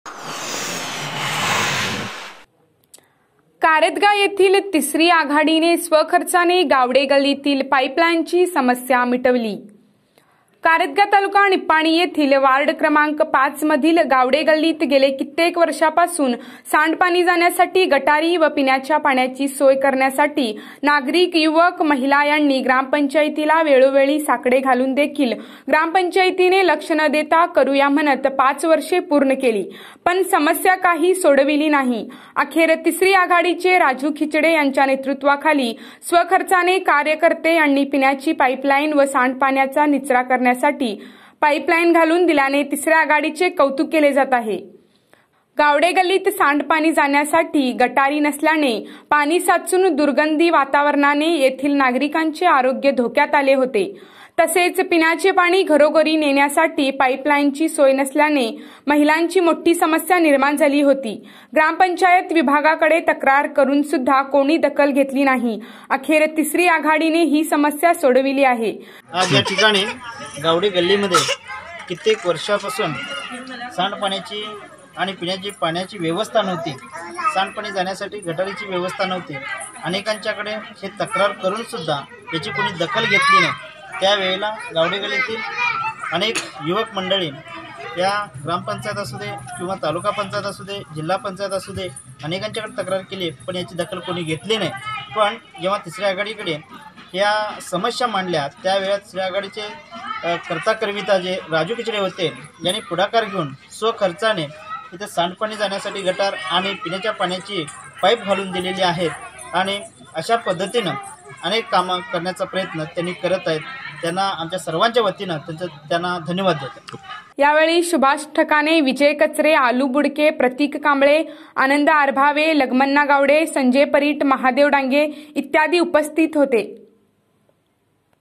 Kardaga कारदगा येथील तिसरी आघाडीने स्वखर्चाने मिटवली पाइपलाइनची समस्या
ICB NEWS KARDAGA